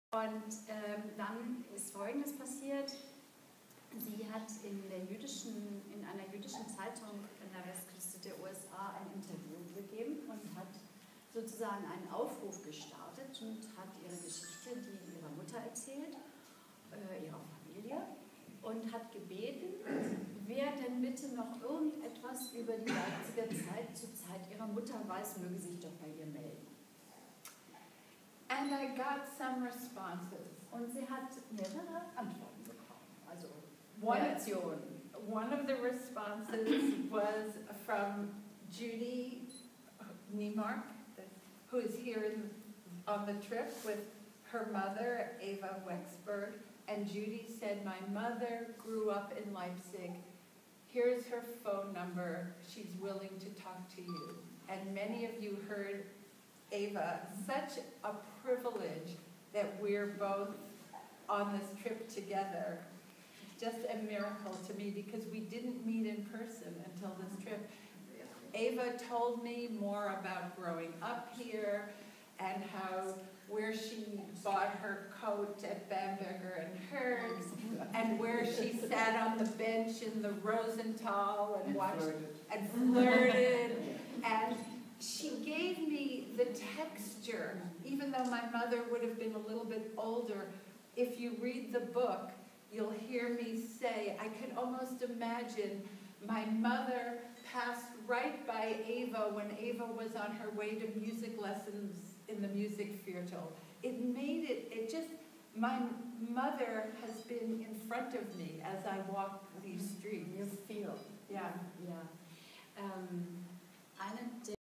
Mani Feniger author in Germany
In the Leipzig city library, Mani describes how she learned more about the texture of her mother's life in Germany by talking to people who grew up around the same time. In this clip, she mentions Eva Wechsburg who is in the audience today.